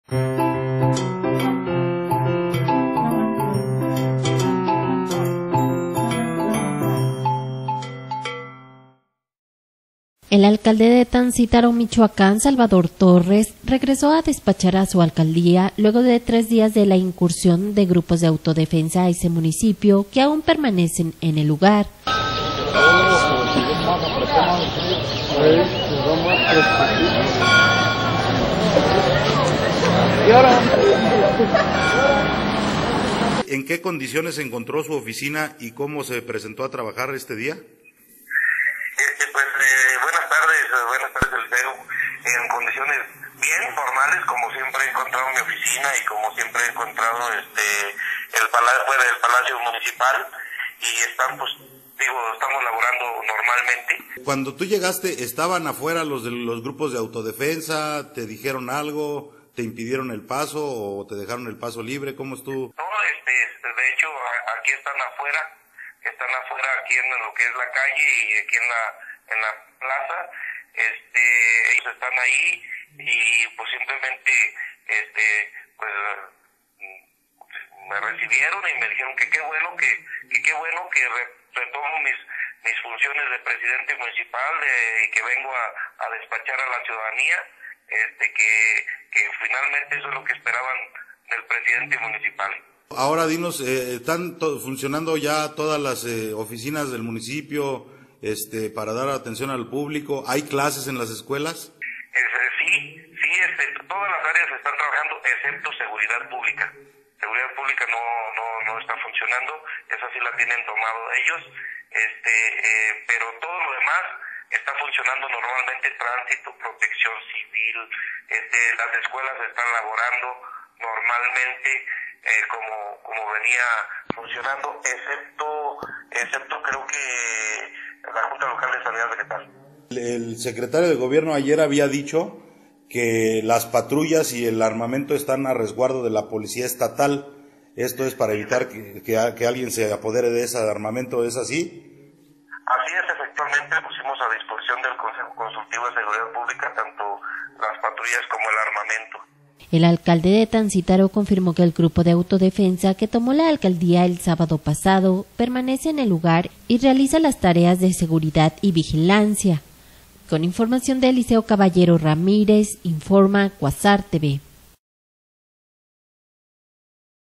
Regresa edil de Tancítaro a despachar la alcaldía
El alcalde de Tancítaro, Michoacán, Salvador Torres, regresó a despachar a su alcaldía, luego de tres días de la incursión de grupos de autodefensa a ese municipio, que aún permanecen en el lugar...